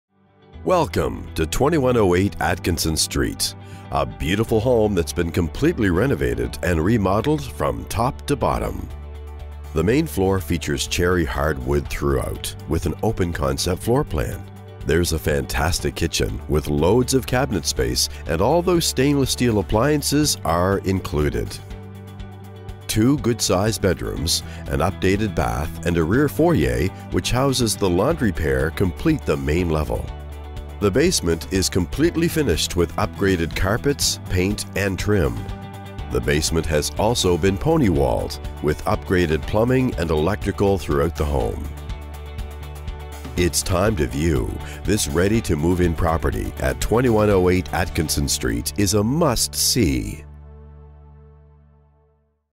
2108 Atkinson Street
Welcome to 2108 Atikinson st. a beautiful home that's completely renovated and remodelled from top to bottom. The main floor features cherry hardwood throughout with an open concept floor plan., fantastic kitchen with loads of cabinet space and stainless steal appliances included. Two good size bedrooms, a updated bath and rear foyer which houses the laundry pair completing the main level. Basement completely finished with upgraded carpets, paint and trim.  Basement has been pony walled, with upgraded plumbing and electrical throughout home. Ready to move in this property is a must see.